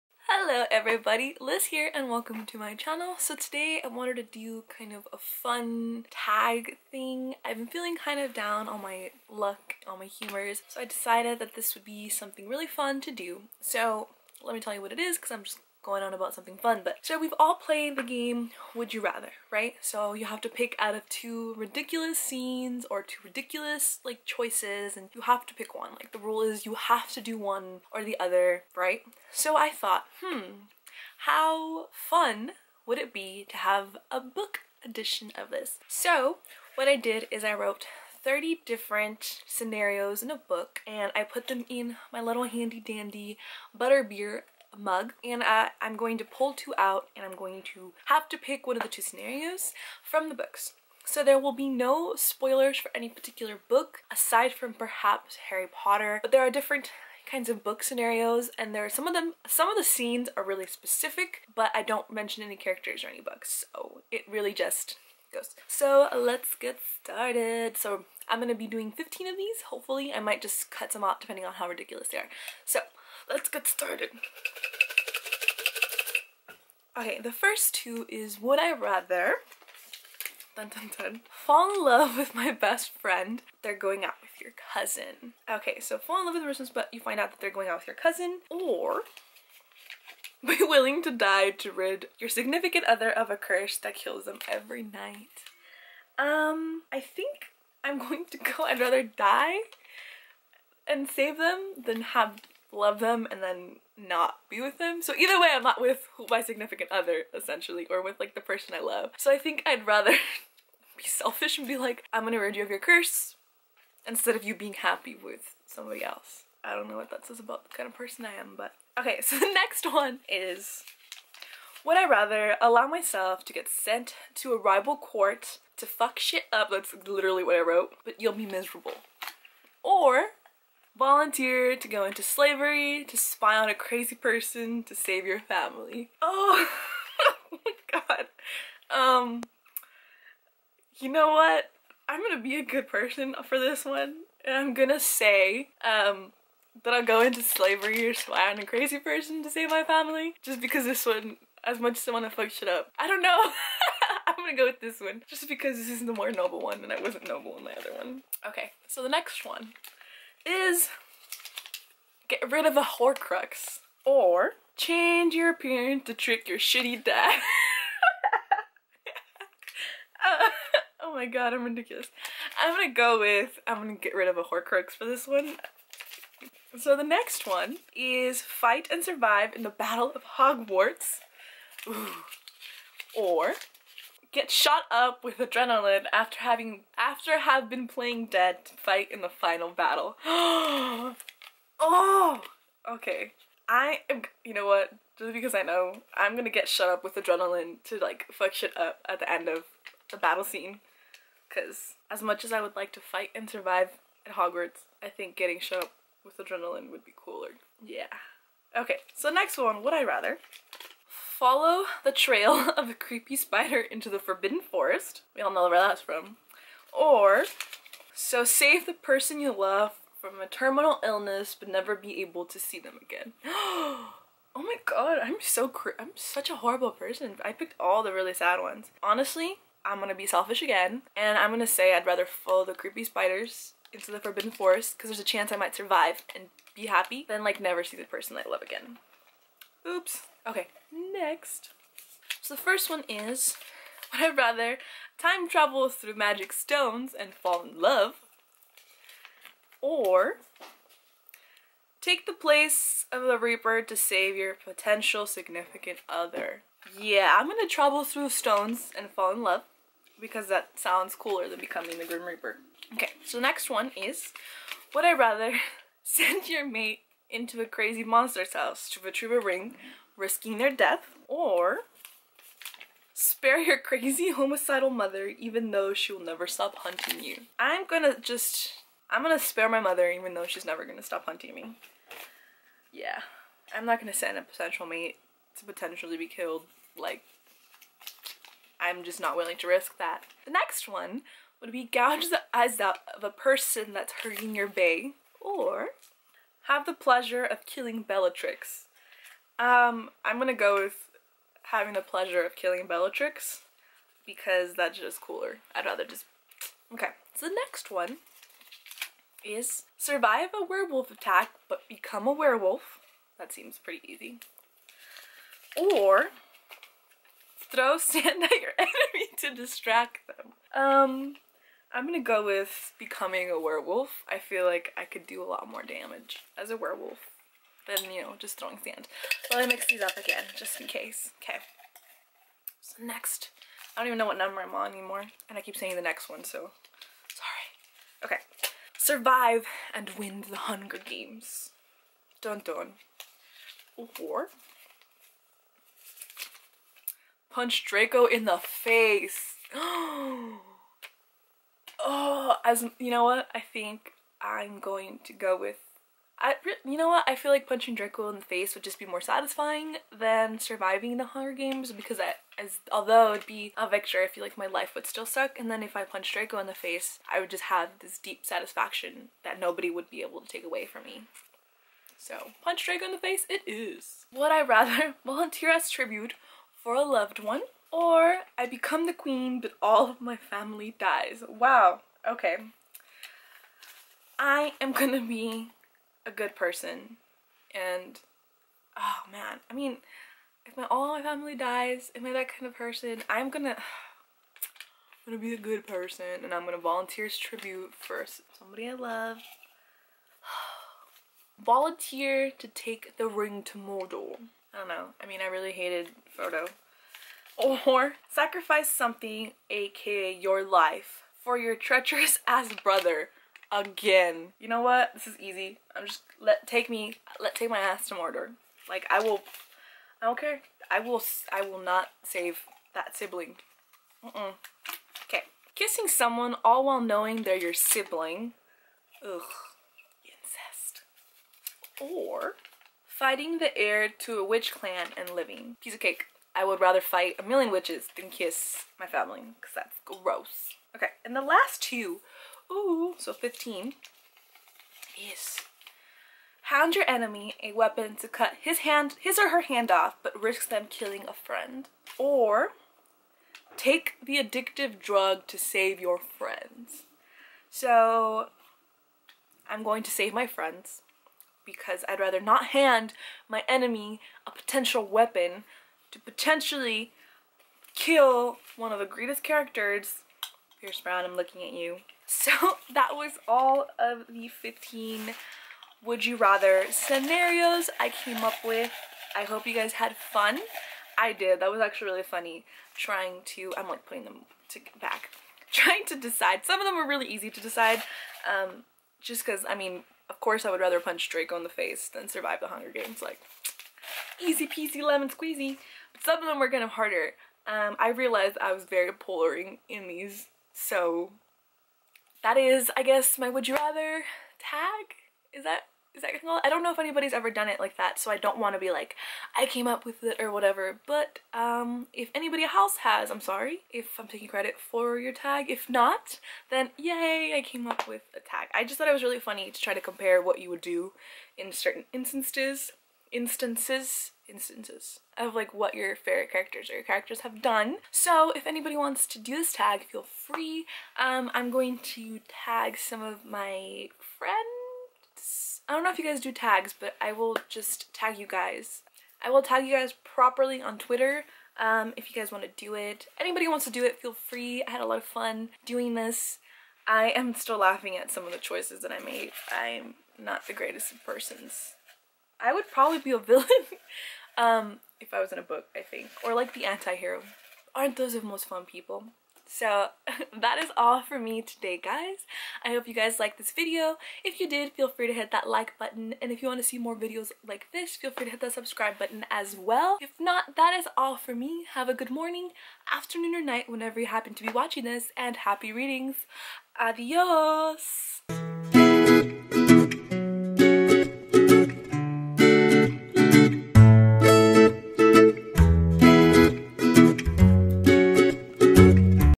Would you Rather: Book Scenarios Tag!
I felt a bit under the weather so I wondered, how fun would it be to play the famous game using book scenarios! Hope you guys like this video.